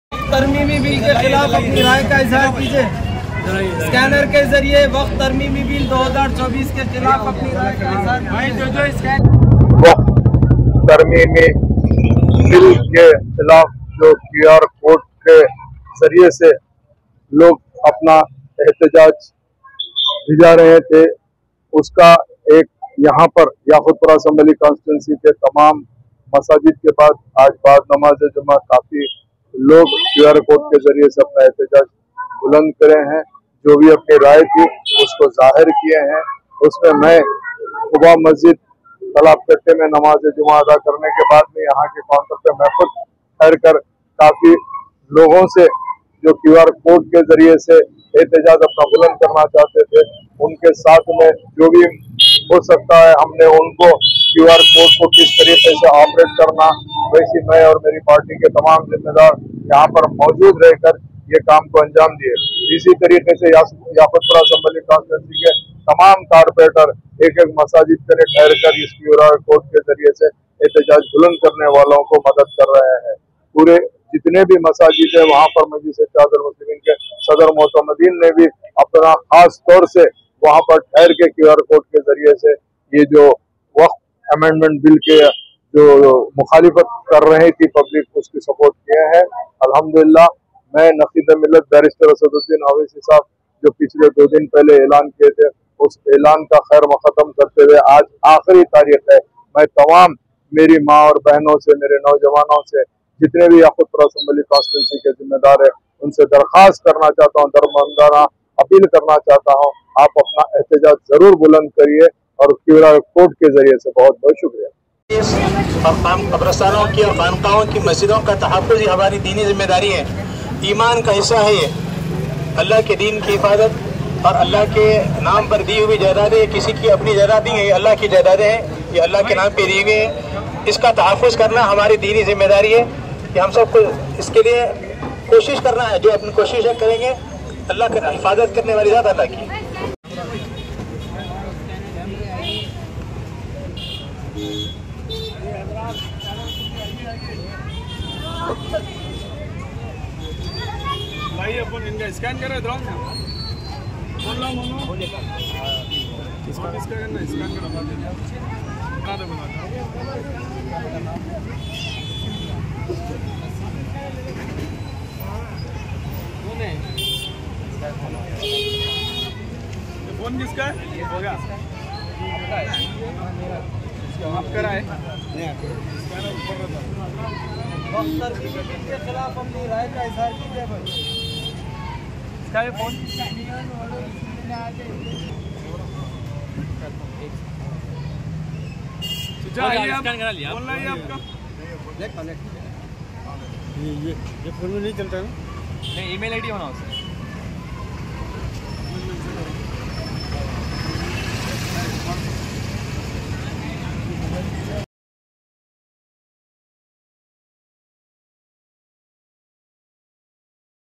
aimim Yakutpura mla Janab Jaffar Hussain Meraj
After offering Namaz E Jummah in Masjid E Quba - Talab Katta, AIMIM Yakutpura MLA Janab Jaffar Hussain Meraj Sahab can be seen Explaining the people how we can register our protest against Waqf Amendment Bill 2024 by using QR Code.